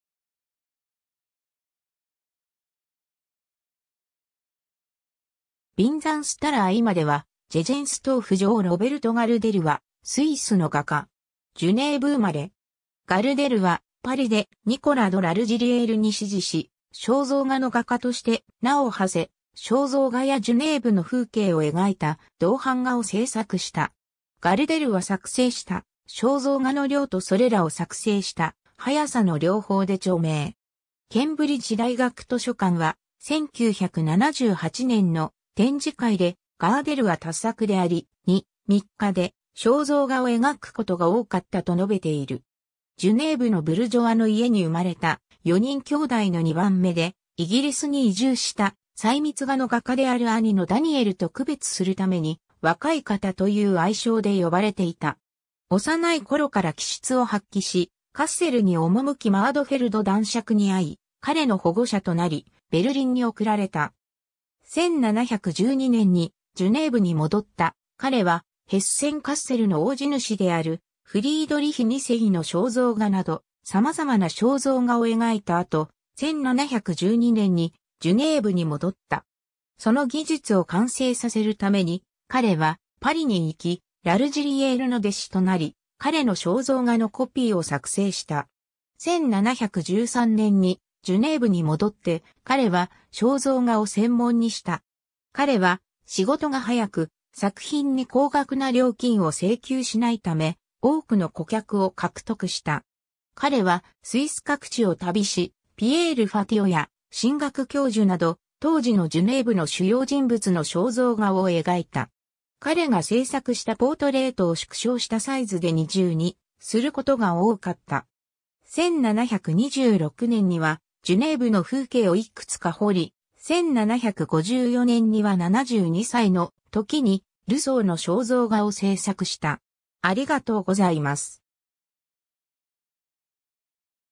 ロベルト・ガルデル
VinzenzStürler今では、 Jegenstorf城 ロベルト・ガルデル（Robert Gardelle、1682年4月9日 - 1766年3月7日）は、スイスの画家。
 ジュネーヴ（当時はジュネーヴ共和国）生まれ。
ガルデルはパリでニコラ・ド・ラルジリエールに師事し、肖像画の画家として名を馳せ、肖像画やジュネーヴの風景を描いた銅版画を制作した。
 ガルデルは作成した肖像画の量とそれらを作成した速さの両方で著名。
ケンブリッジ大学図書館は、1978年の展示会で、ガーデルは多作であり、「2、3日で肖像画を描くことが多かった」と述べている。
ジュネーブのブルジョワの家に生まれた、4人兄弟の2番目で、イギリスに移住した細密画の画家である兄のダニエル（1679年10月2日 - 1753年10月9日）と区別するために「若い方」という愛称で呼ばれていた。
 幼い頃から気質を発揮し、カッセルに赴きマードフェルド男爵に会い、彼の保護者となり、ベルリンに送られた。
1712年にジュネーヴに戻った彼は、ヘッセン・カッセルの大地主であるフリードリヒ2世の肖像画など、様々な肖像画を描いた後、1712年にジュネーヴに戻った。
その技術を完成させるために、彼はパリに行き、ラルジリエールの弟子となり、彼の肖像画のコピーを作成した。
 1713年にジュネーヴに戻って、彼は肖像画を専門にした。
彼は仕事が早く、作品に高額な料金を請求しないため、多くの顧客を獲得した。
彼はスイス各地を旅し、ピエール・ファティオや神学教授など、当時のジュネーヴの主要人物の肖像画を描いた。
彼が制作したポートレートを縮小したサイズ（24×180cm）で二重にすることが多かった。
1726年にはジュネーヴの風景をいくつか彫り、1754年には72歳の時にルソーの肖像画を制作した。